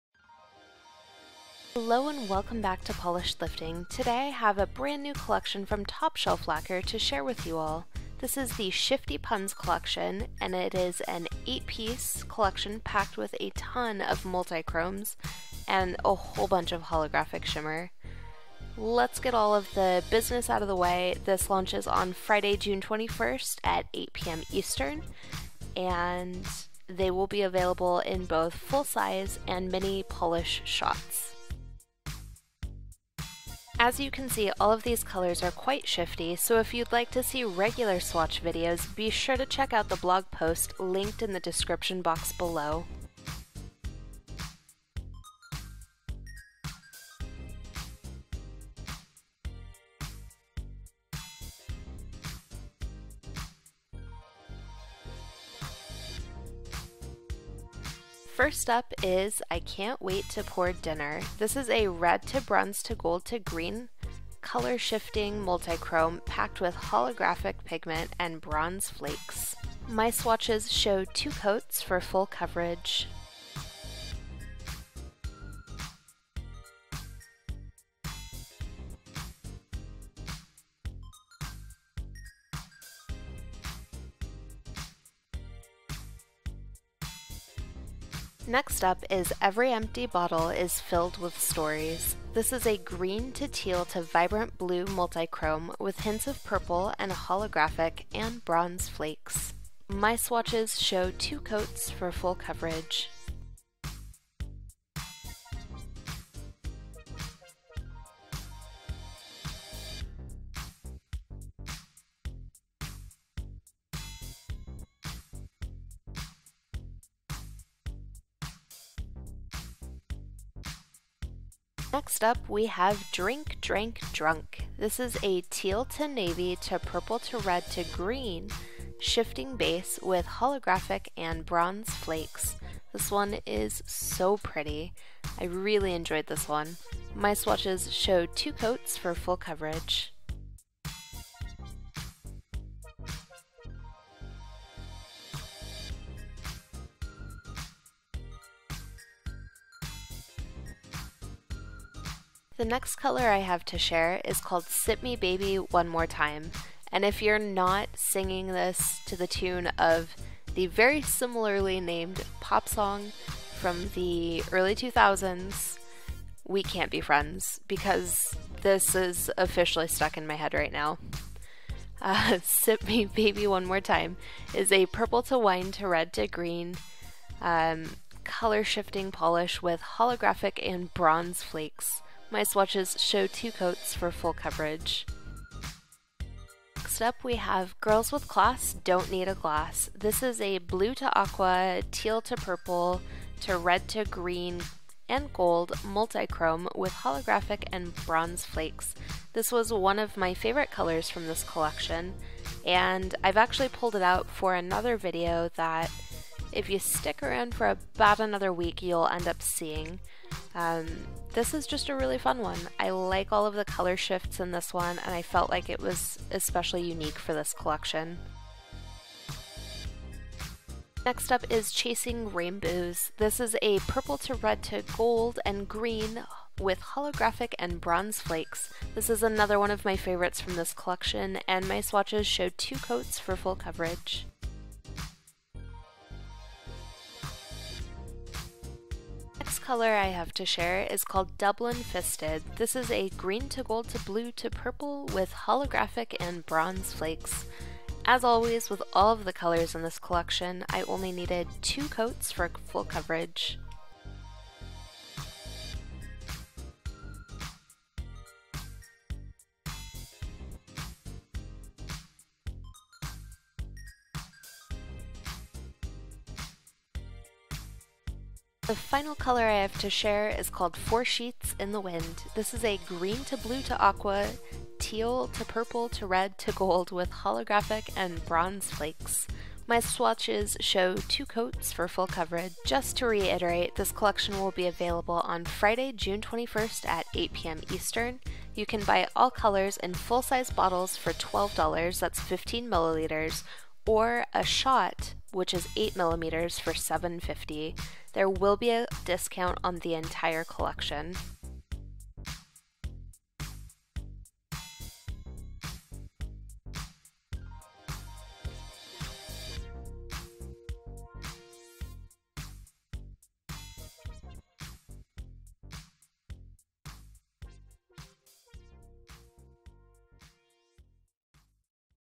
Top Shelf Lacquer Shifty Puns Collection Swatches
Today's video features live swatches of Top Shelf Lacquer Shifty Puns Collection. This launches on Friday, June 21st at 8pm eastern. Each color retails for $12 for a 15mL bottle or $7.50 for an 8mL bottle.

Product List

1. Drink Drank Drunk
2. Chasing Rainbooze
4. Sip me baby, one more time
5. Girls with class, don’t need a glass
6. Every empty bottle is filled with stories
7. Dublin fisted
8. I can’t wait to pour dinner

Important Links